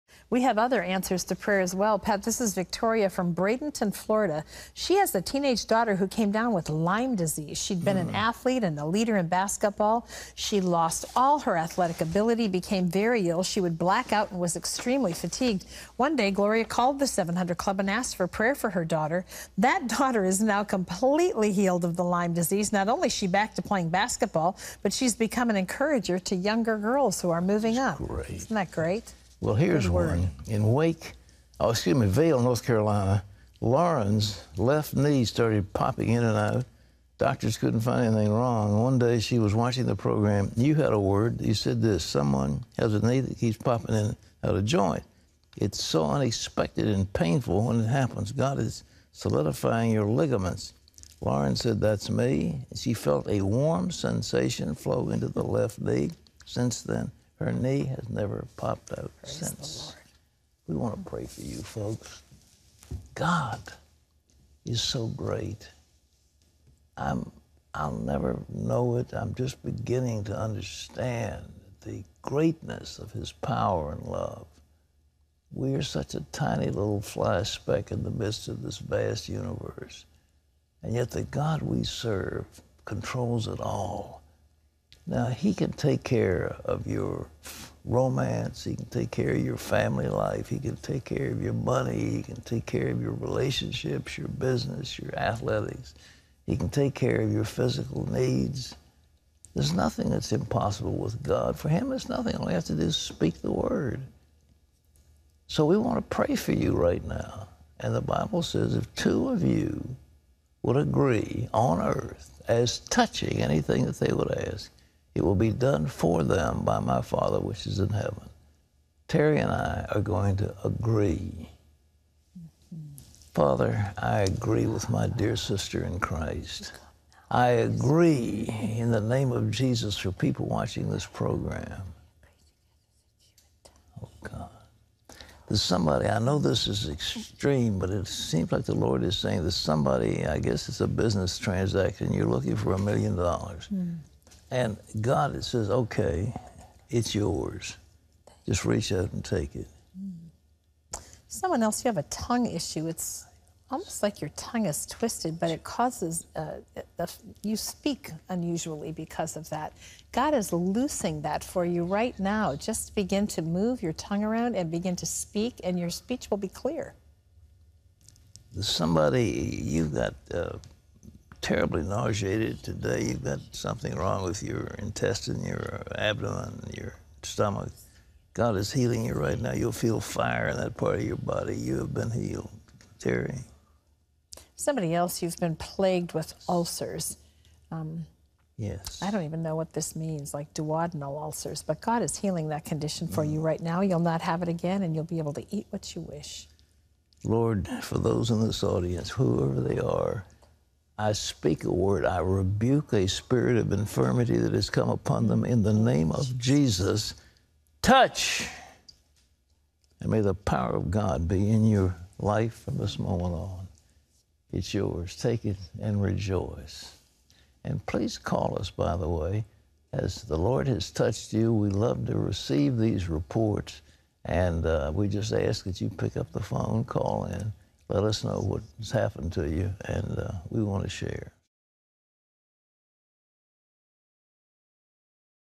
Praying For Your Needs: March 17, 2016
Pat and Terry pray for you and deliver words of knowledge through the power of the Holy Spirit for your healing.